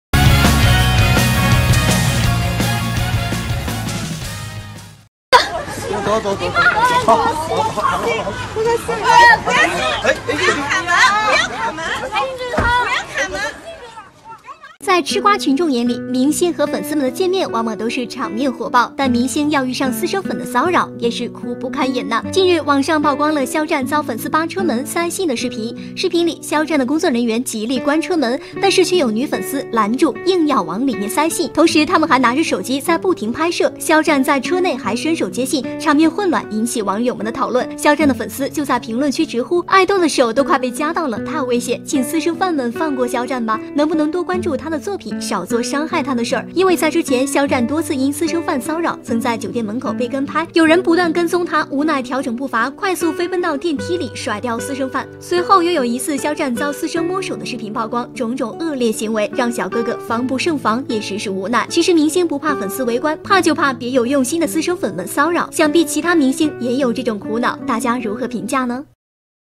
肖战被粉丝堵门摸手后，又被女粉丝扒车门塞信，场面混乱好危险
近日，网上曝光了肖战遭粉丝扒车门塞信的视频，视频里，肖战的工作人员极力关车门，但是却有女粉丝拦住硬要往里面塞信，同时她们还拿着手机在不停拍摄，肖战在车内还伸手接信，场面混乱引起网友们的讨论，肖战的粉丝就在评论区直呼，爱豆的手都快被夹到了，太危险，请私生饭们放过肖战吧！